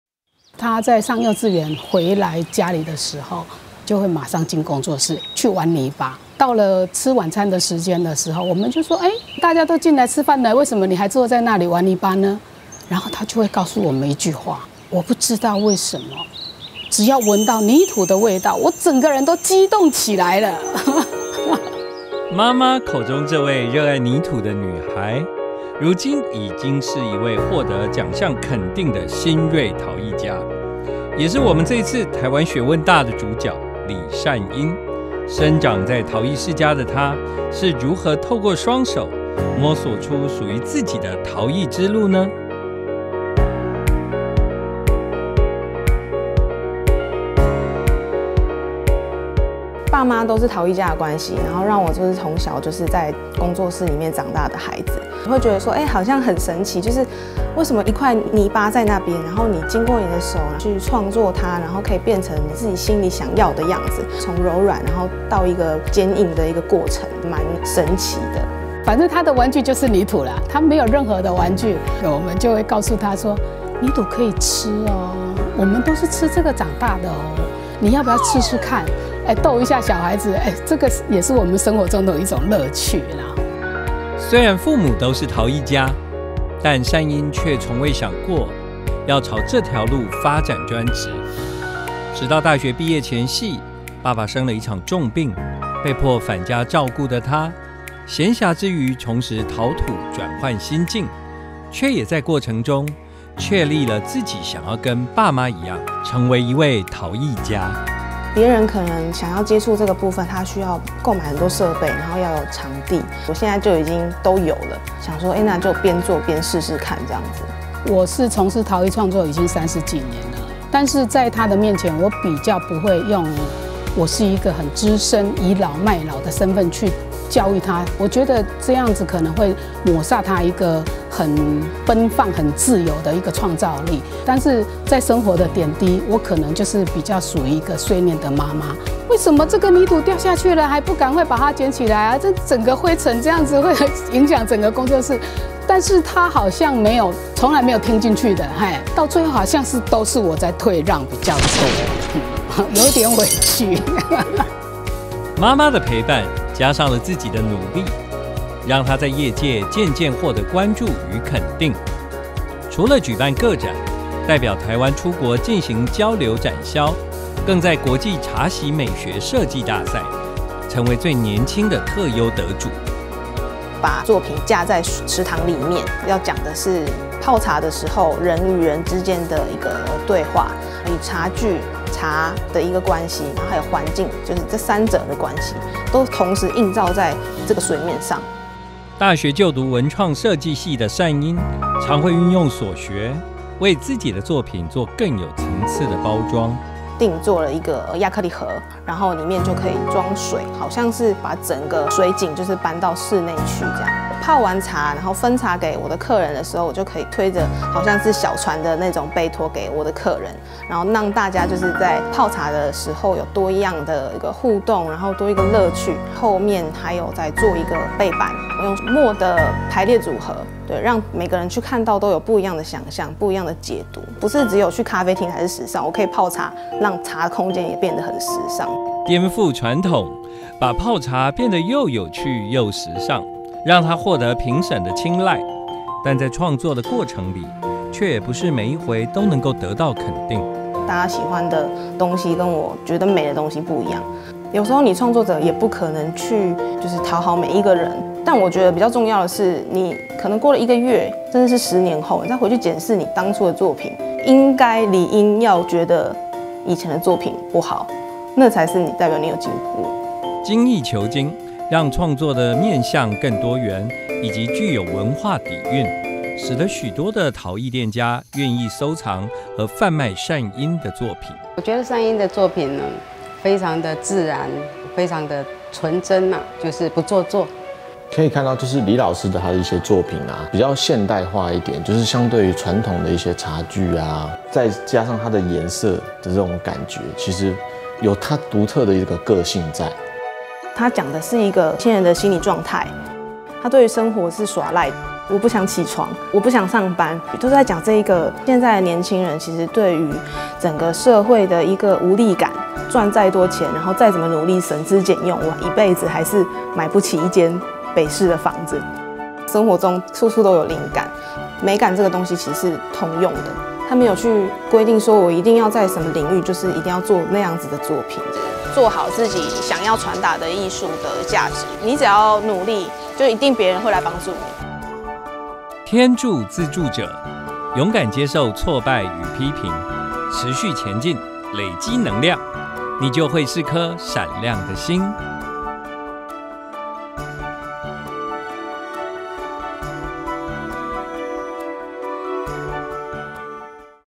【台灣學問大】新銳陶藝家 李善愔－玩土不會讓我吃土！？我的人生由我做主！！
【台灣學問大】X 新銳陶藝家 李善愔

台灣學問大 帶您探訪台灣各地職人精神，
細細品味屬於他們的奮鬥故事！
今天要跟著 陶藝家李善愔 的腳步！
體會她用自己的雙手摸索出來的陶藝之路！